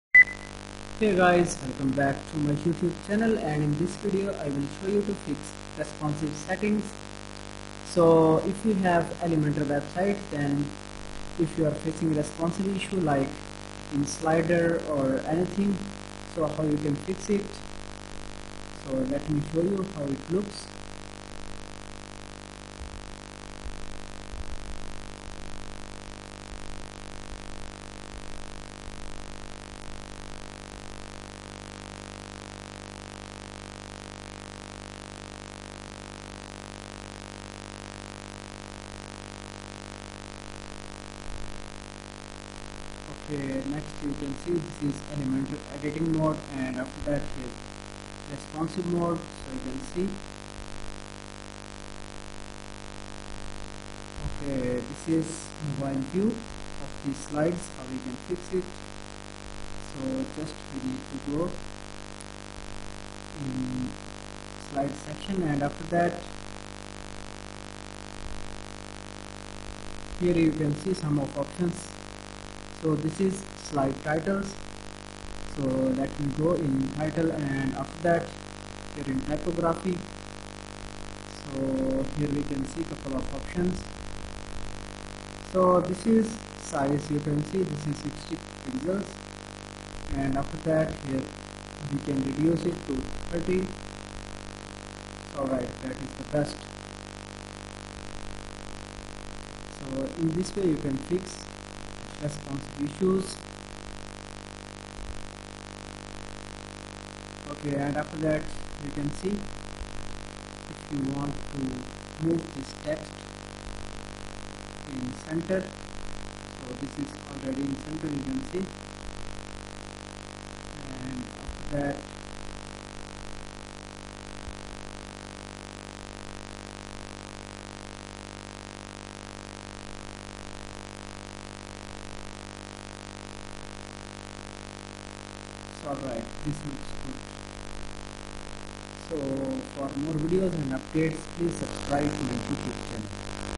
How to fix reponsive website design issue in elementor
1. Check the page settings: Make sure that the page layout is set to “Responsive” in the Page Settings.

2. Check the widget settings: Make sure that the widgets you are using are also set to “Responsive” in their settings. This will ensure that they are displayed properly on different devices.

3. Apply custom styling: If you are still having issues, you can always apply custom styling to the elements to make them display properly on different devices.

4. Use breakpoints: Elementor allows you to set breakpoints at different resolutions, so you can design your page to look great on any device.

5. Use the Preview mode: You can use the Preview mode to test how your page looks on different devices, so you can make any necessary changes.